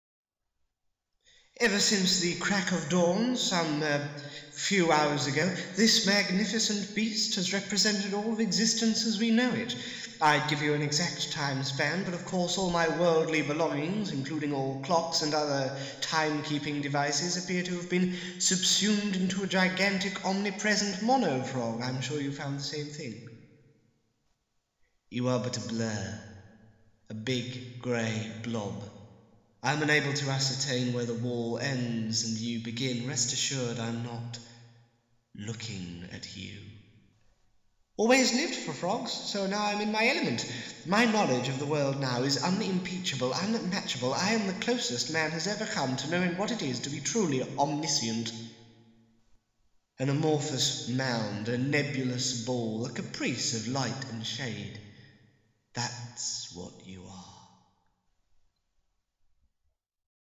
Amplexus: An Amphibious Duet - Trailer
Fresh from the Edinburgh Fringe, Fast Parson Theatre presents 'The Curious Incident of the Frog in My Sightline' ("An absurdist whirlwind of furrowed brows and hearty chuckles. ★★★★" - A Younger Theatre) and its never-before-seen companion piece, 'Alphalotl and Olmega', in 'AMPLEXUS: An Amphibious Duet' at the London Theatre, New Cross, appearing for one night only as part of the Lewisham Fringe Festival.